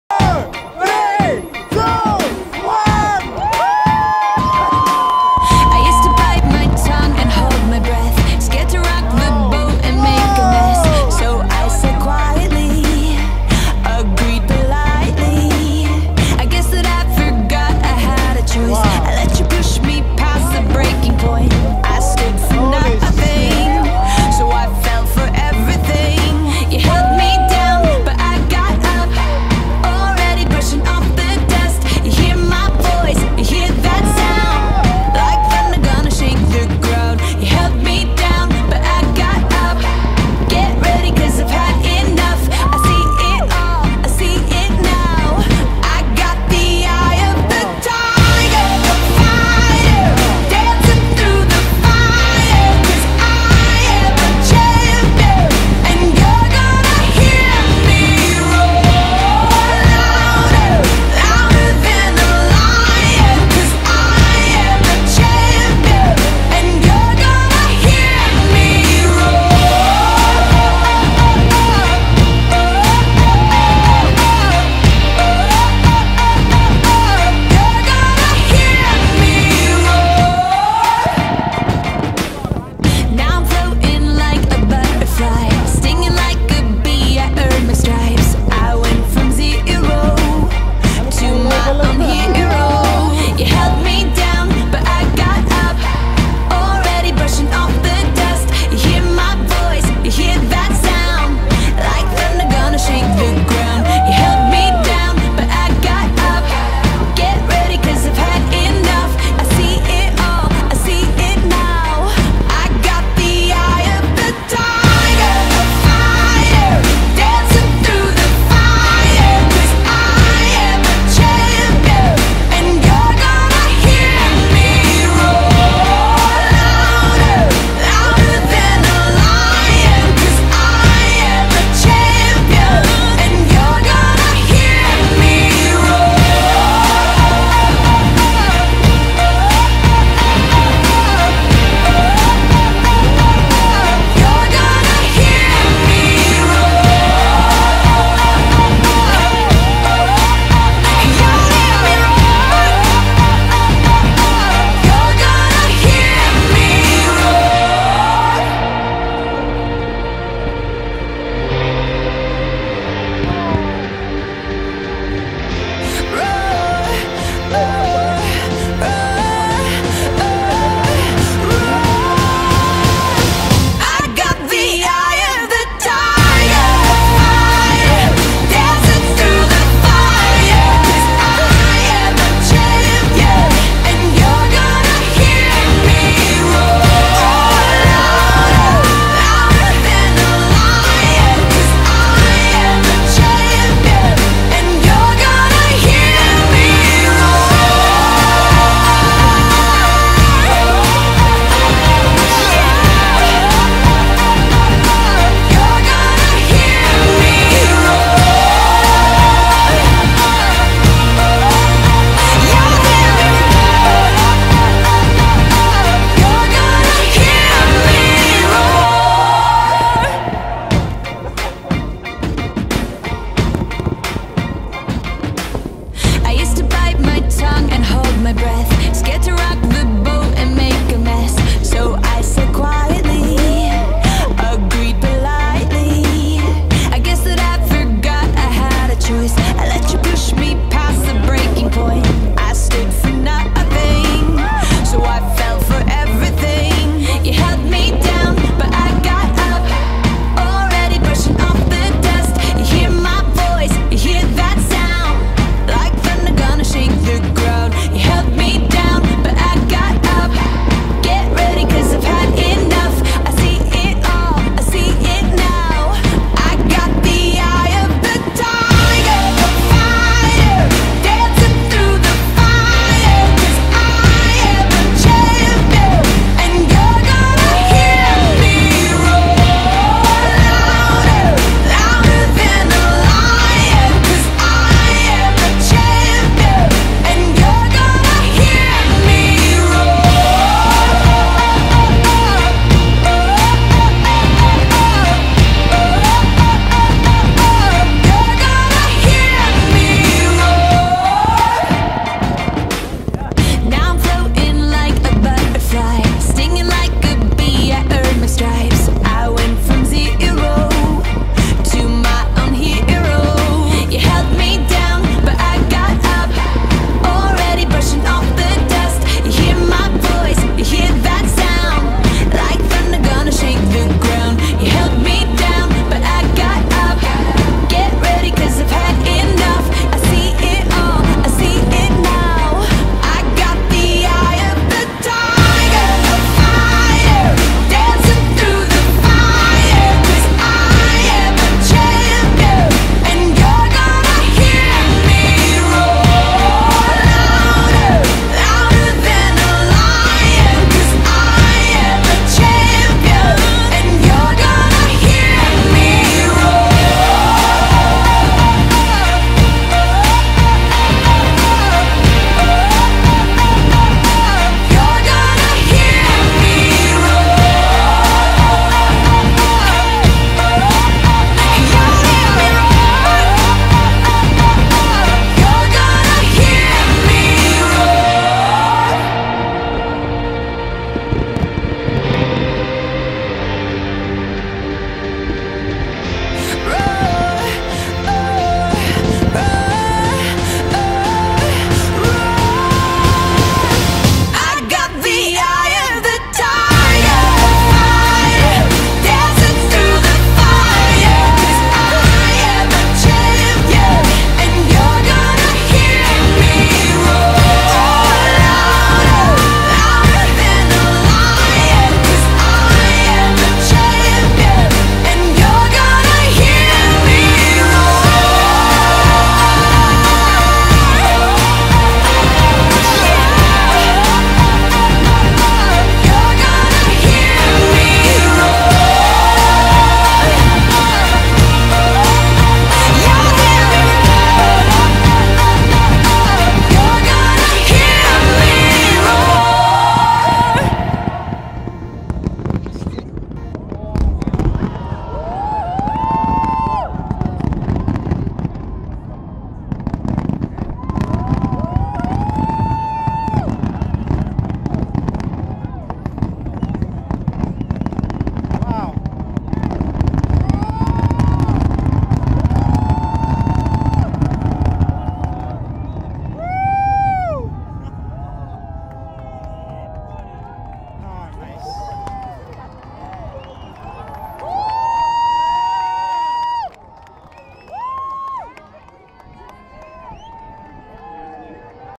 New Year Fireworks @ Marina Bay, Singapore
New Year Fireworks, view from SkyPark - Marina Bay Sands

Taken with a Nokia Lumia 1020